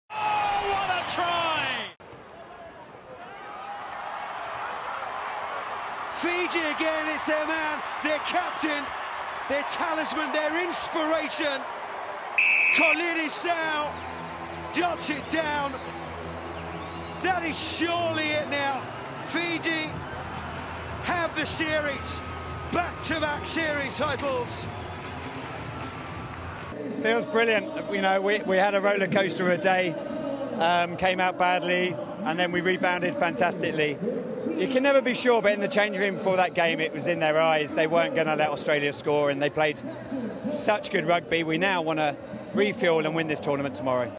Fiji claim World Series title
Fiji have beaten Australia to claim the World Series for 2015/16 season, and remain strong favourites for Gold in Rio.